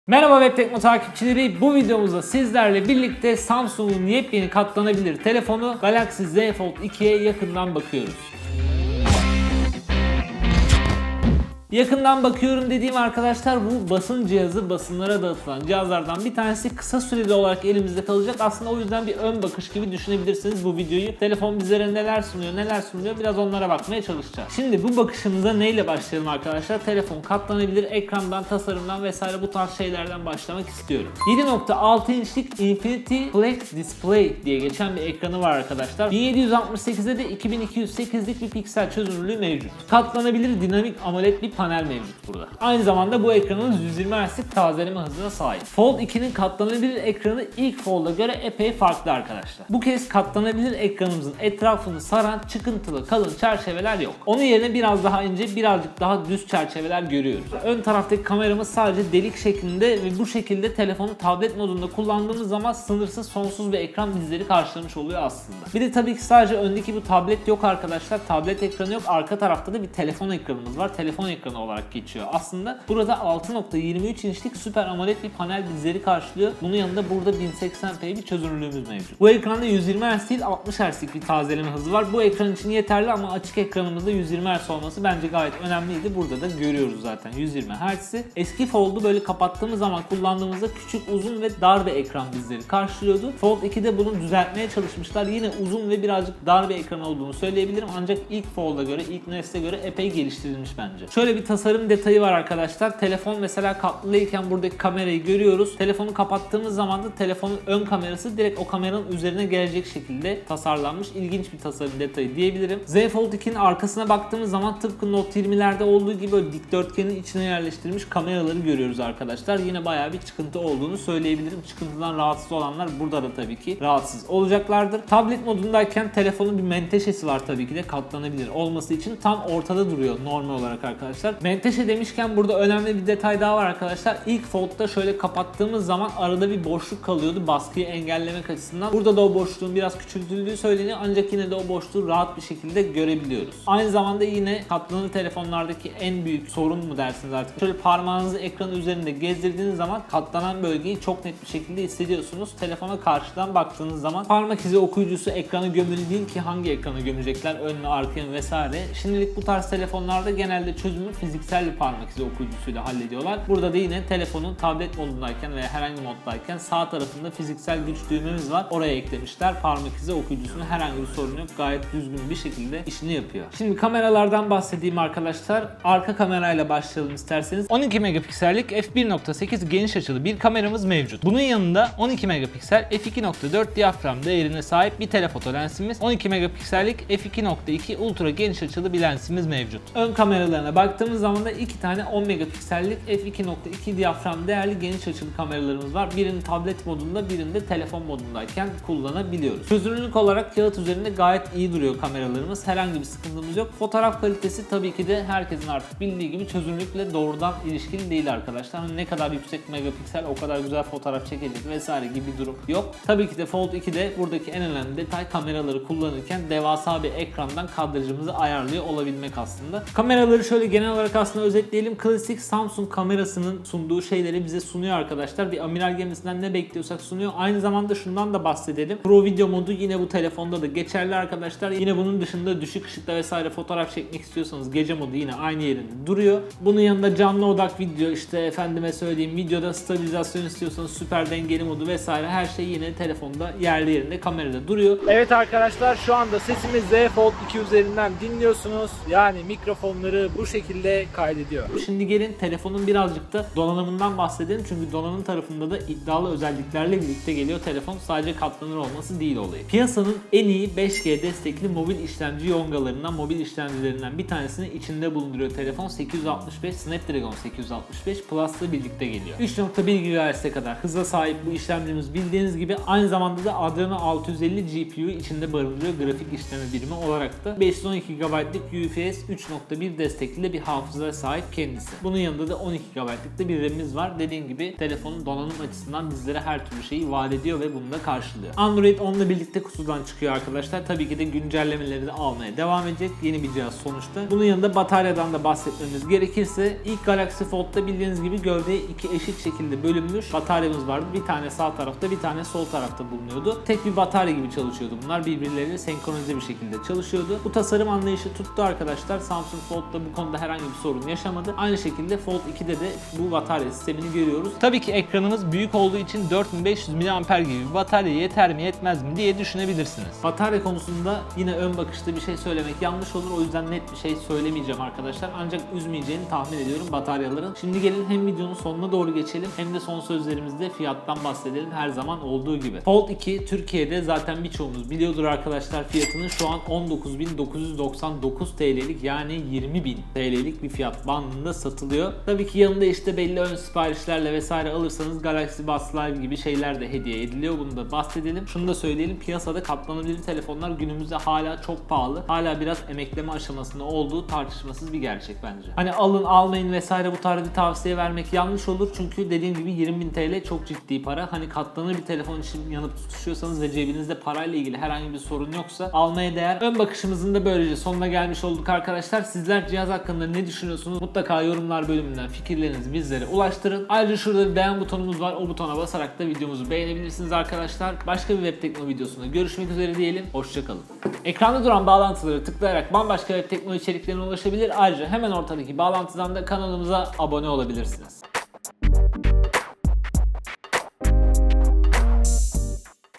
Bize Katlanabilecek mi? #3 - Samsung'un Katlanan Yeni Telefonu: Galaxy Z Fold 2 Ön Bakış
Merhaba Webtekno takipçileri,

Katlanabilir telefon konusunda sektörün öncü isimlerinden Samsung, Galaxy Fold ve Galaxy Z Flip ardından yeni telefonu Galaxy Z Fold 2’yi Türkiye'de de resmi olarak tanıttı. Daha önceki iki telefon ile edindiği tecrübeyi Galaxy Z Fold 2’ye aktaran Samsung, bu kez daha etkileyici bir telefonla karşımıza çıktı.
2019’da ilk katlanabilir telefonu Galaxy Fold ile bir tableti telefon boyutunda sunan Samsung, daha sonra bir telefonu cüzdan boyutuna getirdiği tasarımıyla Galaxy Z Flip’i tanıtmıştı. Her iki telefon da katlanabilir ekranlarla mümkün olabilecek deneyimlerin büyük bir kısmını bize sundu. Ancak teknolojide imkanlar tükenmiyor. Samsung tablet ve telefon özelliği taşıyan yeni katlanabilir telefonu Galaxy Z Fold2’yi Türkiye'de resmi olarak tanıttı.

Bu kez daha güçlü ve sağlam bir katlanabilir ekran ve daha büyük telefon ekranı ile karşımıza çıkan hibrit cihaz, ilk Galaxy Fold’dan aldığı mirası yaşatıyor.

Galaxy Z Fold2’nin katlanabilir ekranı Infinity Flex, ilk Galaxy Fold’a kıyasla oldukça farklı. Bu kez katlanabilir ekranın çevresini saran çıkıntılı ve kalın çerçeveler yok. Onlar yerine düz ve daha ince çerçeveler bizi karşılıyor. Ayrıca ilk Galaxy Fold’un katlanabilir ekranın sağ üst köşede yer alan ön kamera çıkıntısı da yerini ekrana bırakmış durumda. Bir başka deyişle Galaxy Z Fold2’nin tablet modunda kullandığımız ekran, artık neredeyse pürüzsüz ve simetrik bir deneyim sunuyor.

İlk Galaxy Fold’da bulunan 7,3 inçlik katlanabilir ekran, Galaxy Z Fold2’de 7,6 inç boyutuna yükselmiş durumda. Infinity Flex adı verilen bu ekranın çözünürlüğü 1768 x 2208 piksel. Bir başka deyişle Full HD+’ın biraz üzerinde bir kaliteye sahip.